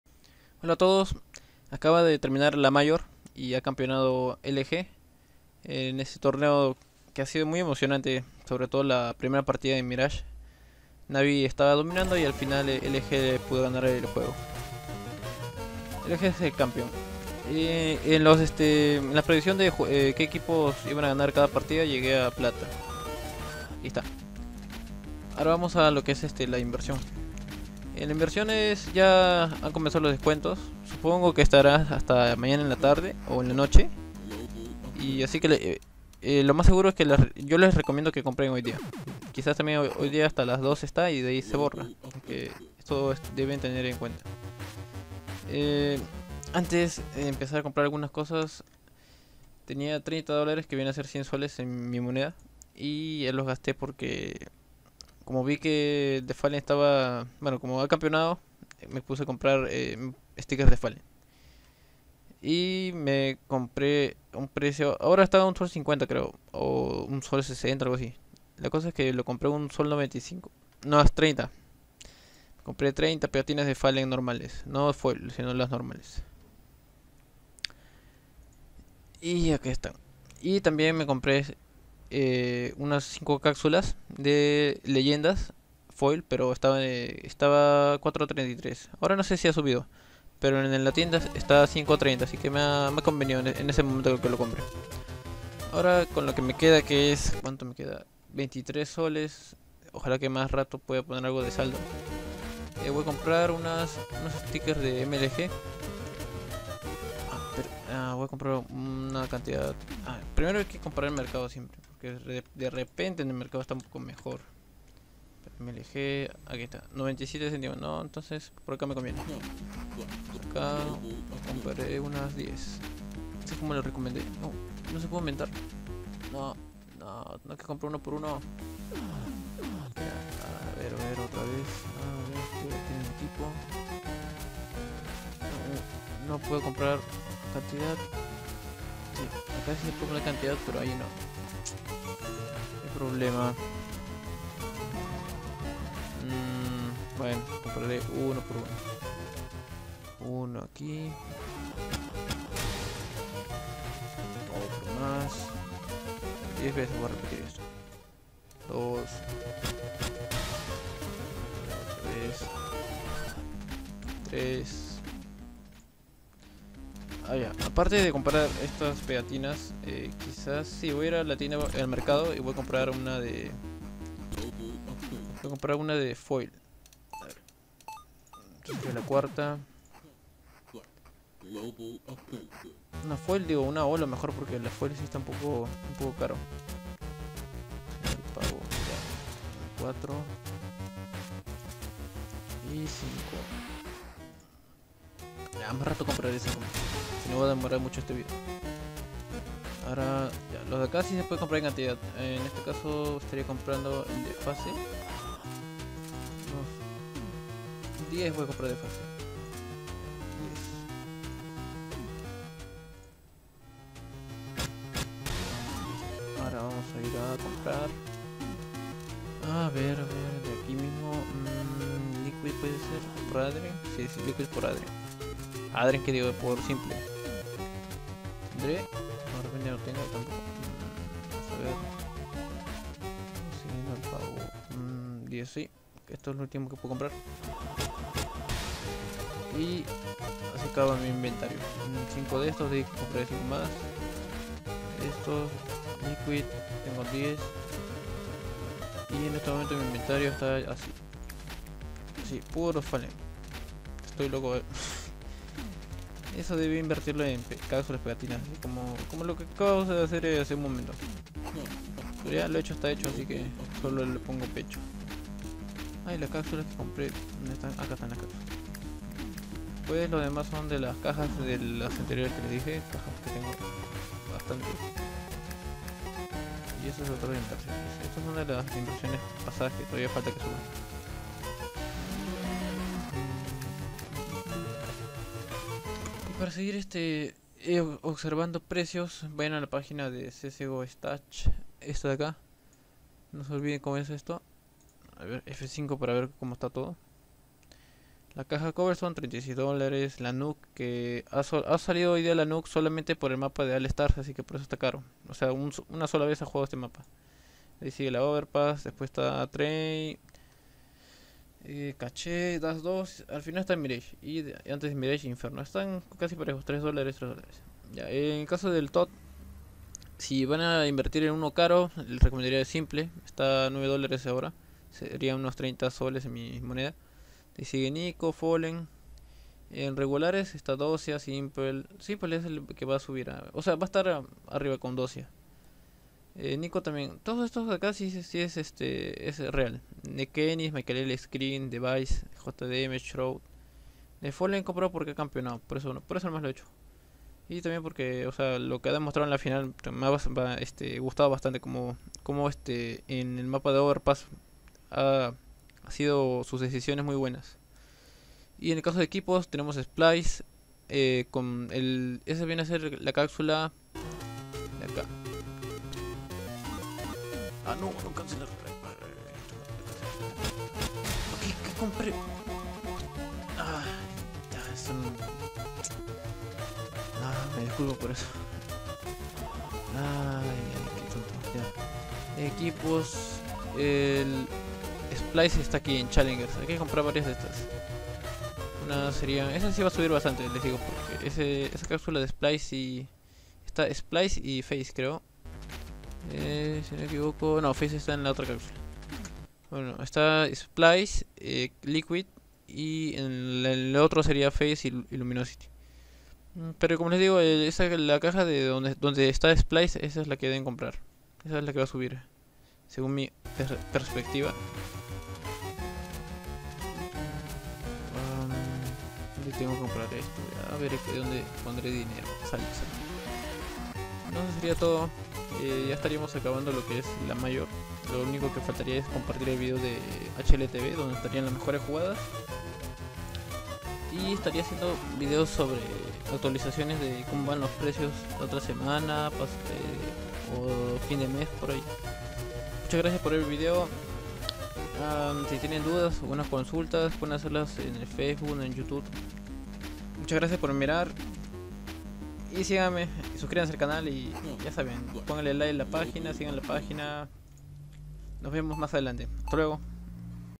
Inversion - Post Major con Descuentos
Es momento de comprar los stickers, capsulas, foil, holo, etc.

He cometido un error :v y ya compré muchas de Fallen (30) más adelante quizás compre en caso tenga saldo ya que gasté comprando las cajas en las otras inversiones. Habrá un vídeo con actualizaciones de las inversiones más adelante, cerca de fin de mes.